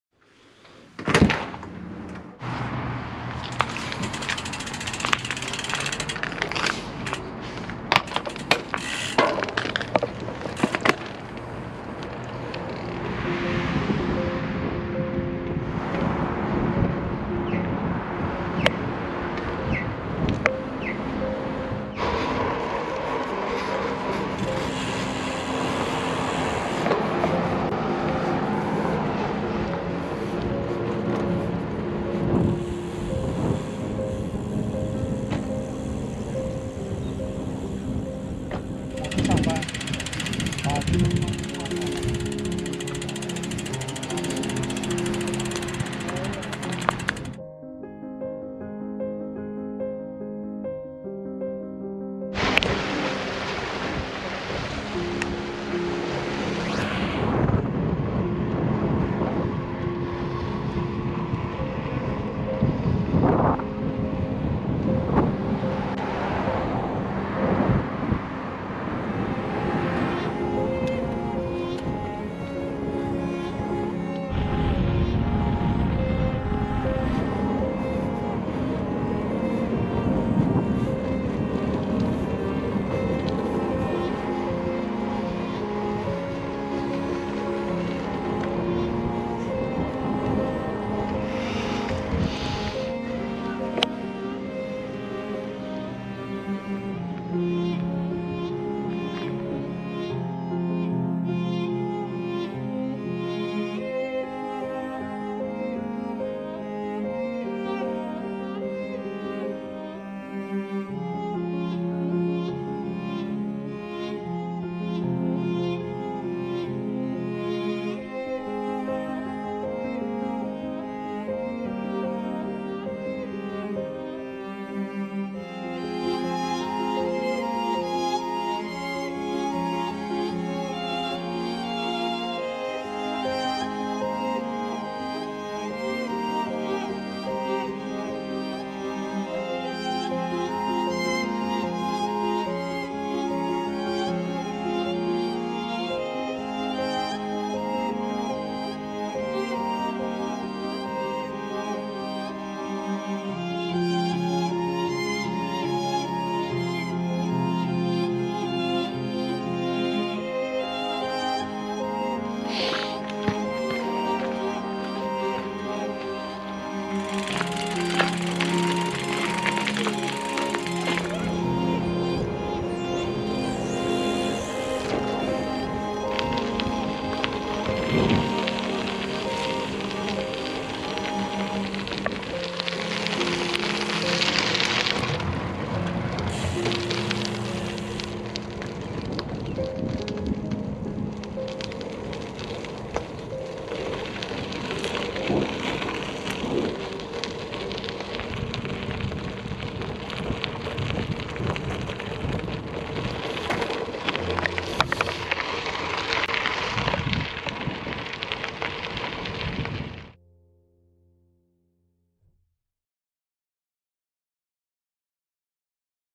Cinematic Victoria by Bike
Testing my videography skills by making a bike ride as cinematic as I can. I'm proud of how this video turned out, and I hope you enjoy seeing some of my favourite spots around Victoria, BC.

Shot on a GoPro Hero 8, with a GoPro Media Mod. Filmed primarily in 2.7k, 24fps, high bit rate, and protune custom settings. The drone I use is a DJI Mavic Mini, I shoot in 2.7k and 25fps.